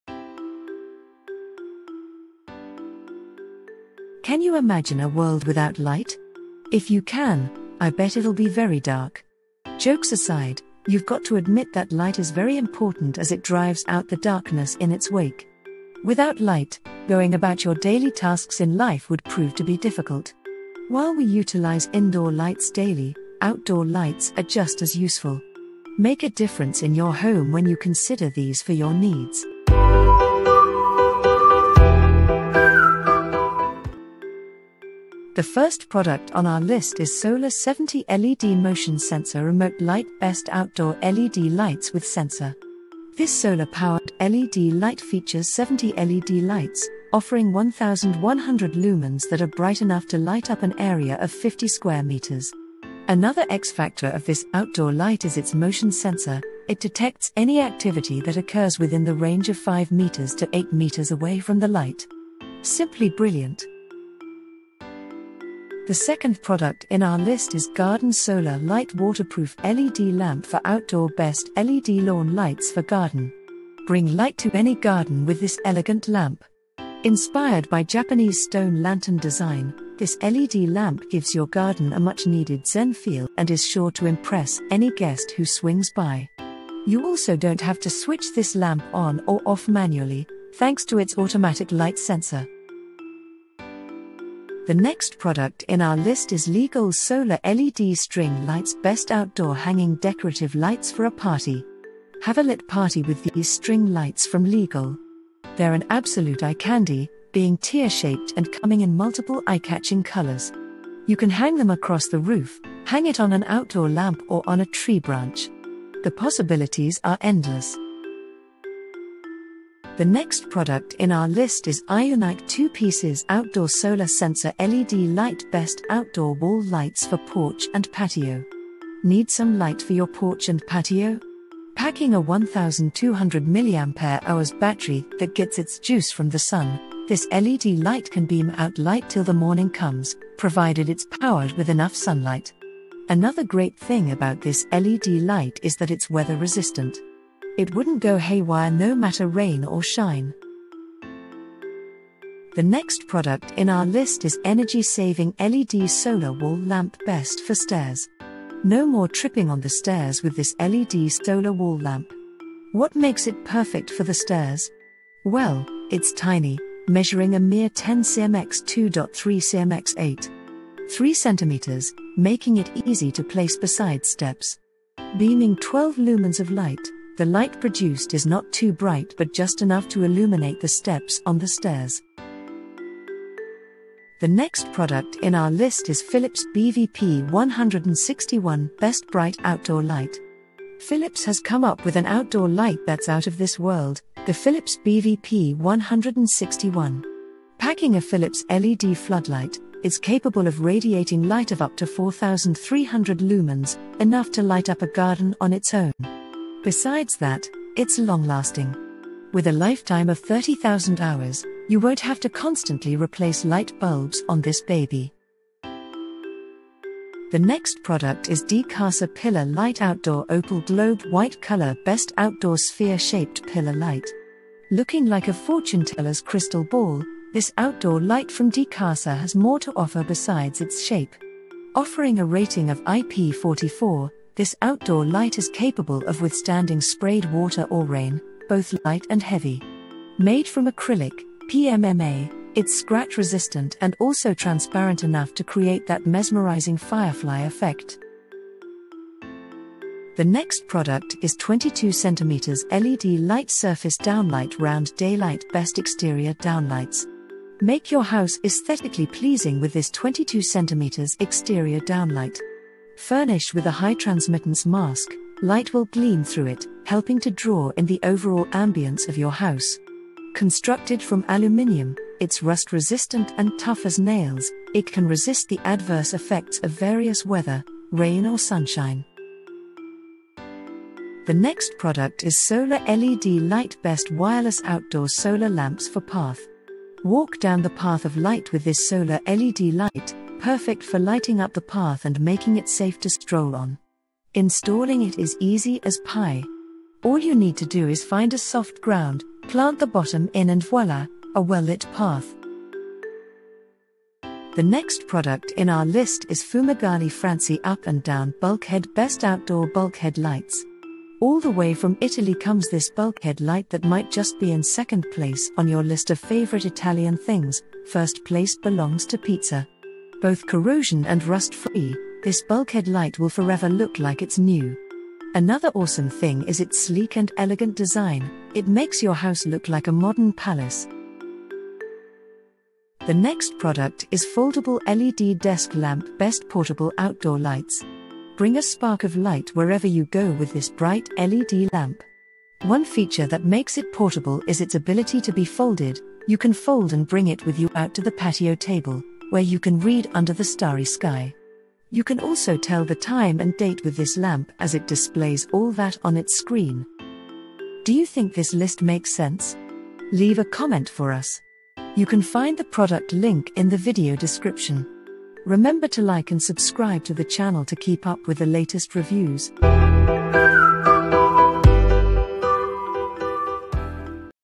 🌵 11 Best Outdoor Lighting in Malaysia
00:00 Introduction
00:33 #1 - Solar 70 LED Motion Sensor Remote Light Best outdoor LED lights with sensor (RM107.80)
01:03 #2 - Garden Solar Light Waterproof LED lamp for outdoor Best LED lawn lights for garden (null)
01:32 #3 - Leegoal Solar LED String Lights Best outdoor hanging decorative lights for a party (RM30.32)
01:57 #4 - IUNYK 2pcs Outdoor Solar Sensor LED Light Best outdoor wall lights for porch and patio (null)
02:30 #5 - Energy Saving LED Solar Wall Lamp Best for stairs (null)
03:02 #6 - Philips BVP161 Best bright outdoor light (RM150.50)
03:35 #7 - DECASA Pillar Light Outdoor Opal Globe White Colour  Best outdoor sphere-shaped pillar light (RM52.50)
04:08 #8 - 22cm LED Light Surface Downlight Round Daylight Best exterior downlights (RM14.00)
04:39 #9 - Solar LED Light Best wireless outdoor solar lamps for path (RM45.00)
05:02 #10 - Fumagalli Francy Up And Down Bulkhead Best outdoor bulkhead lights (RM135.00)
05:30 #11 - Foldable LED Desk Lamp Best portable outdoor lights (RM43.72)
05:56 Ending

Can you imagine a world without light? If you can, I bet it’ll be very dark. Jokes aside, you’ve got to admit that light is very important as it drives out the darkness in its wake. Without light, going about your daily tasks in life would prove to be difficult.
While we utilise indoor lights daily, outdoor lights are just as useful. Make a difference in your home when you consider these for your needs.